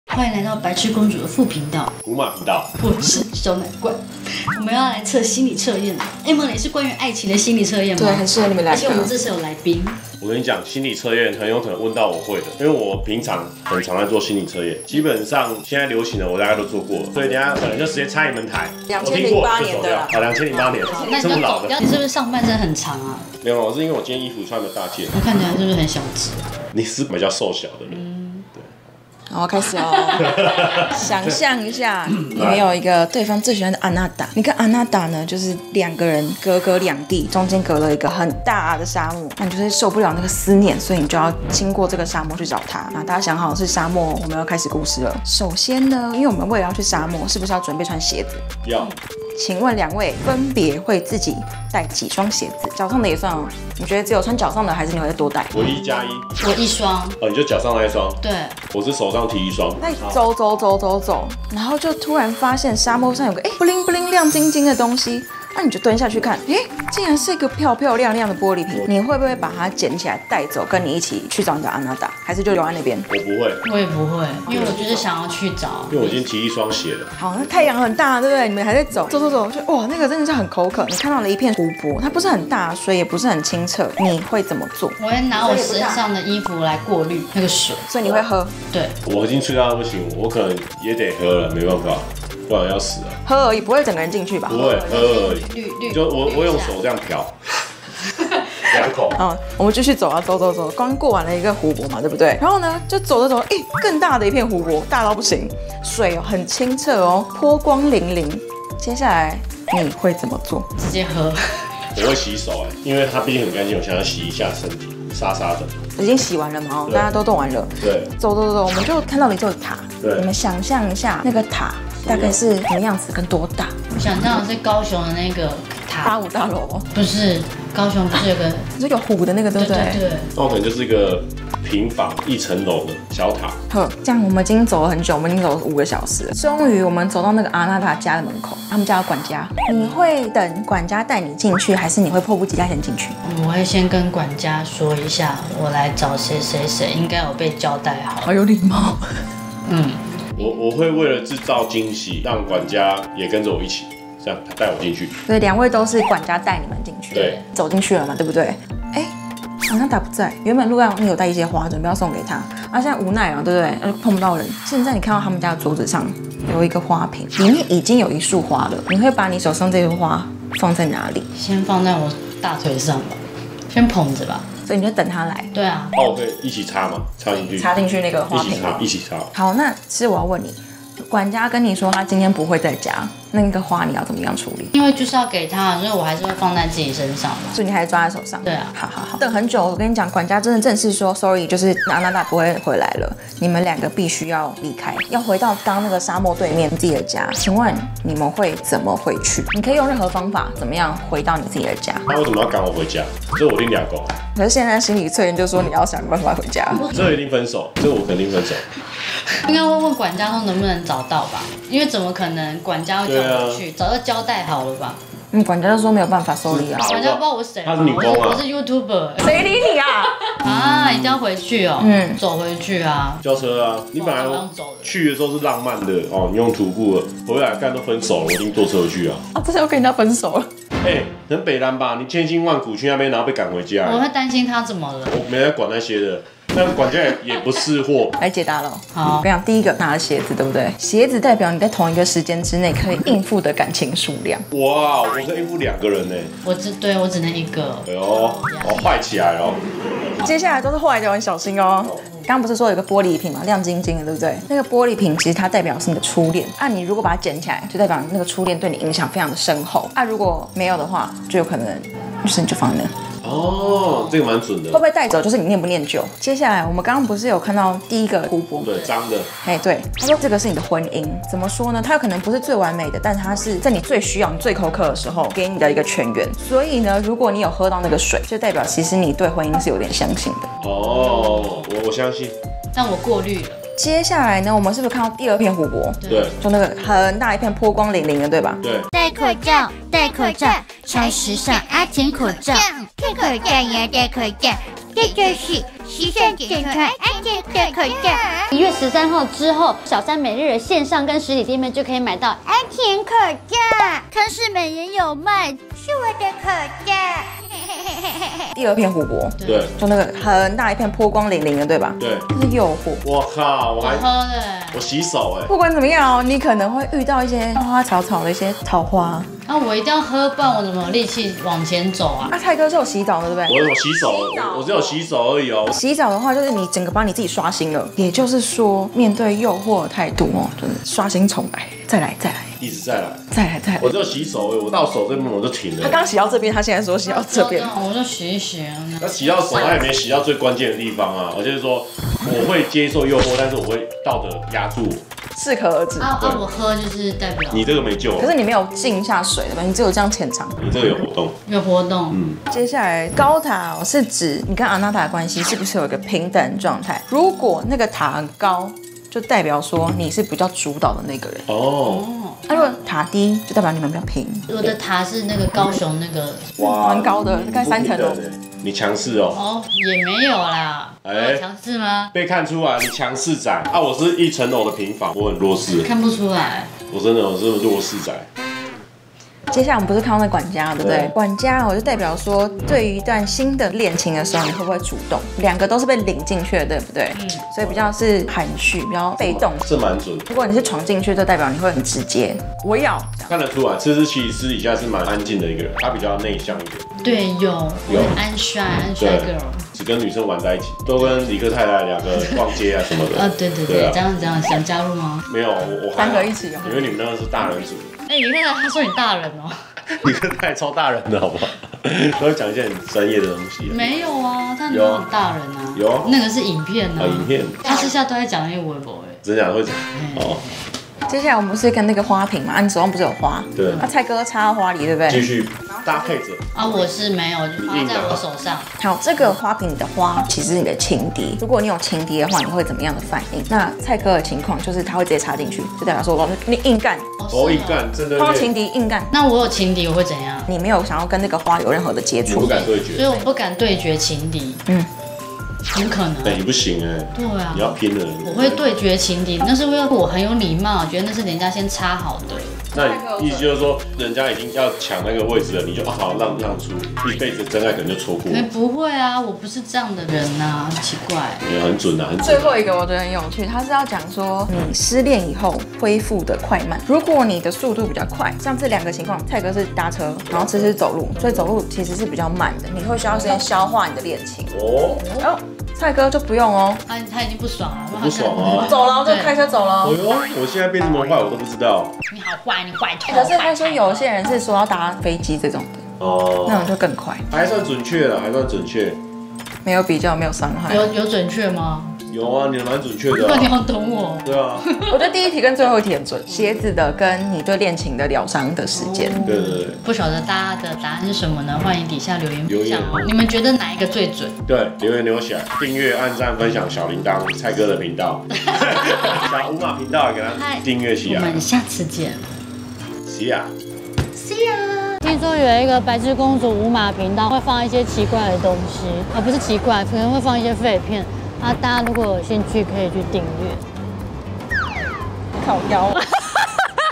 愛情心理測驗！其實白癡公主被動內向不敢主動追求！？超反差！【白癡公主的無碼專區】ft.@coolmantsai
主頻道@​白癡公主
【第二波阿甜口罩的販售點】
➤小三美日 網站也補貨囉
有寄海外：香港澳門馬來西亞新加坡日本美國中國泰國印尼越南